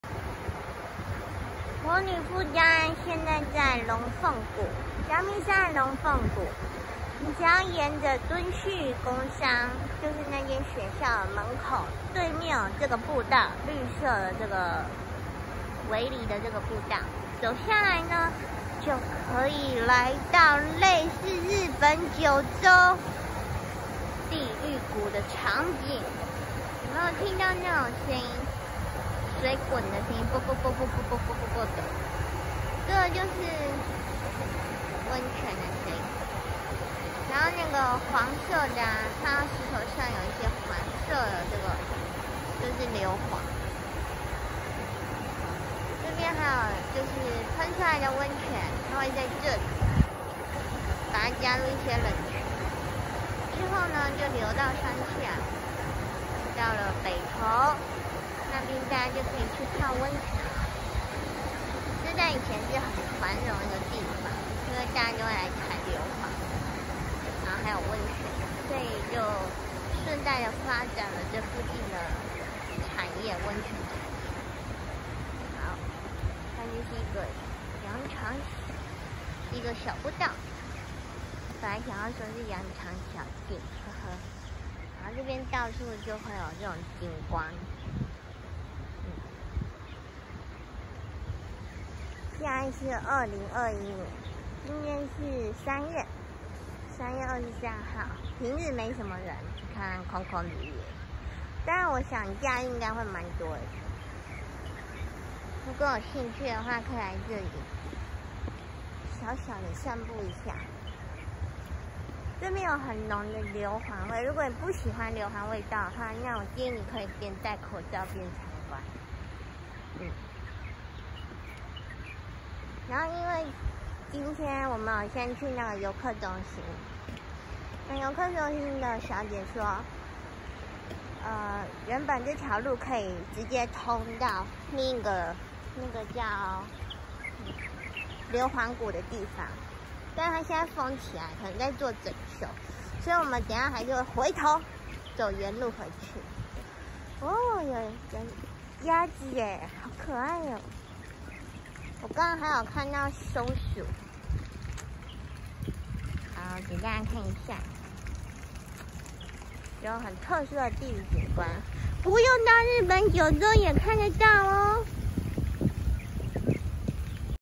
負江直擊 | 陽明山 龍鳳谷獨特硫磺景觀
類似日本九州地獄谷，熱騰騰硫磺溫泉♨️景觀就在陽明山國家公園龍鳳谷😱